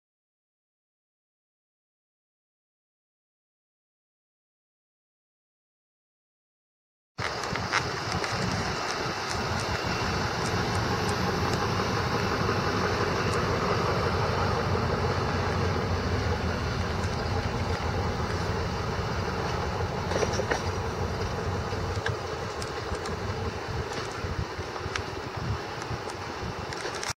2006 Ford Taurus SE for sale | sold at auction October 16, 2014
2006 Ford Taurus SE
145,164 miles on odometer
3.0L V6 OHV 12V FFV gas engine
Automatic transmission
AC and heat
Tilt
Cruise
AM/FM/cassette
Cloth interior
16" wheels
Dead battery
Hail damage
1FAFP53226A210463